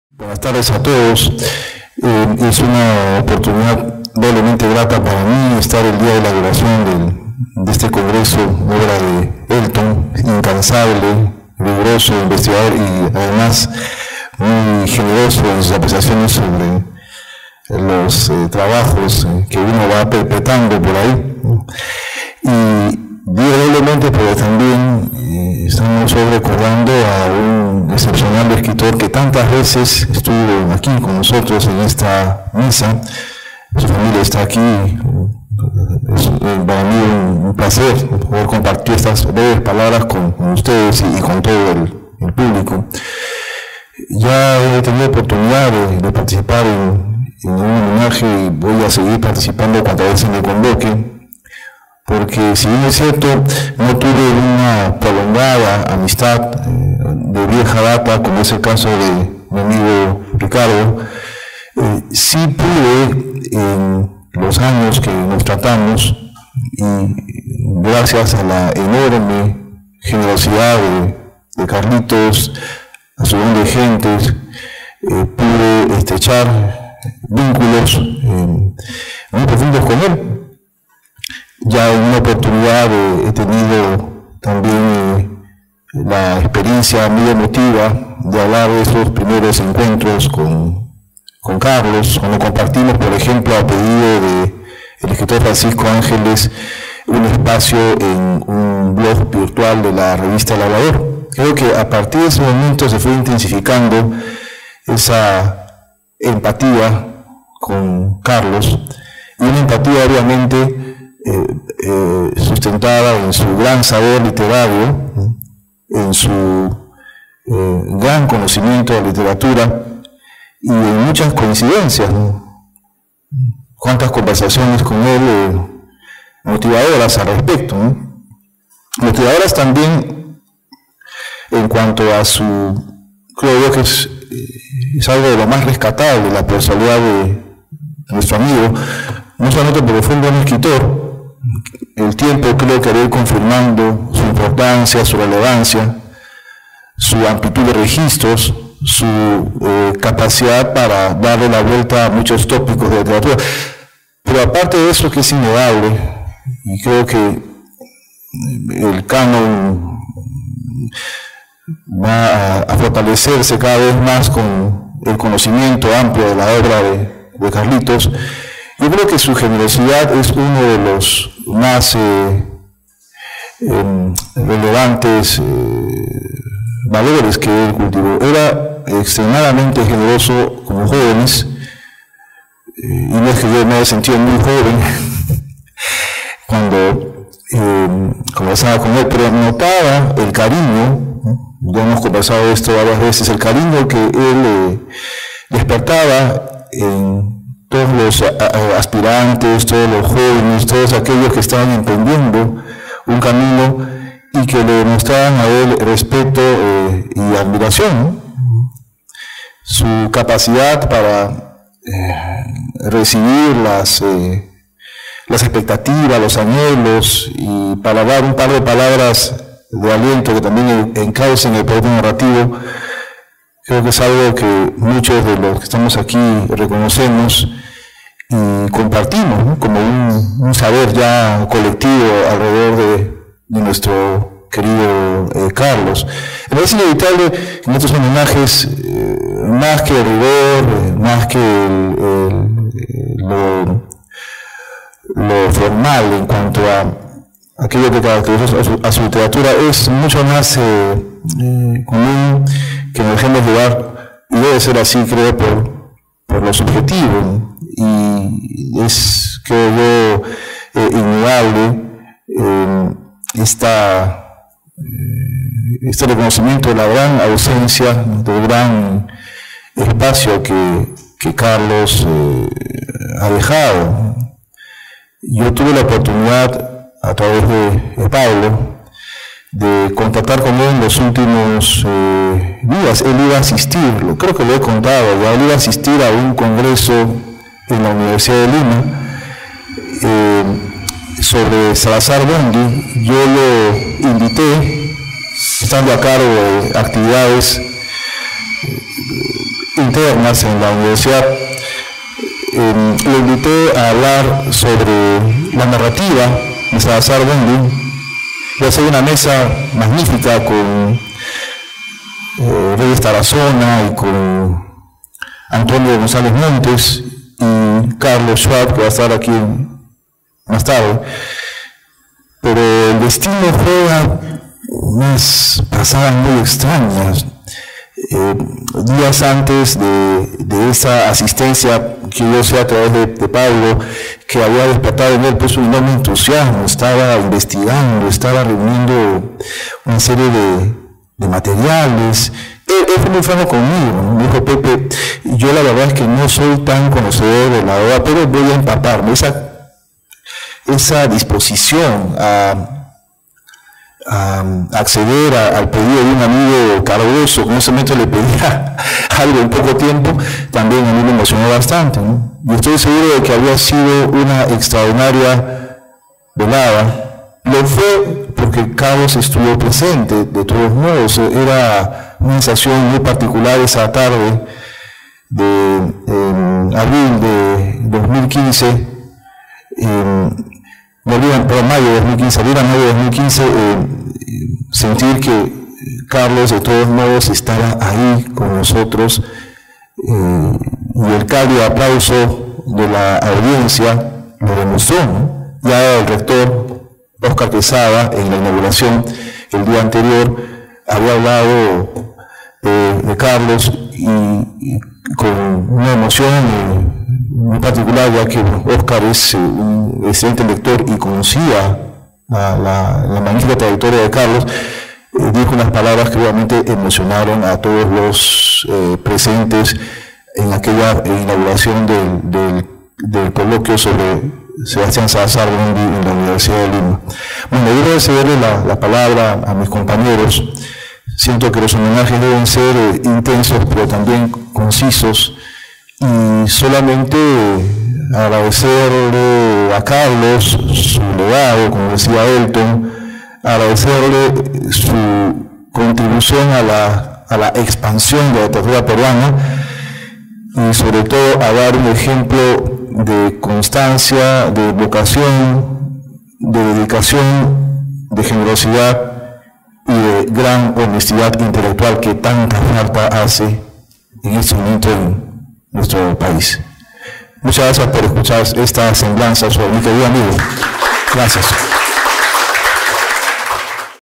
Homenaje a Carlos Calderón Fajardo: José Güich
Homenaje a Carlos Calderón Fajardo en el VI Congreso Nacional de Escritores de Literatura Fantástica y Ciencia Ficción Peruana, organizado por Elton Honores y el Instituto Raúl Porras Barrenechea. Casa de la Literatura Peruana, 25 de febrero de 2016.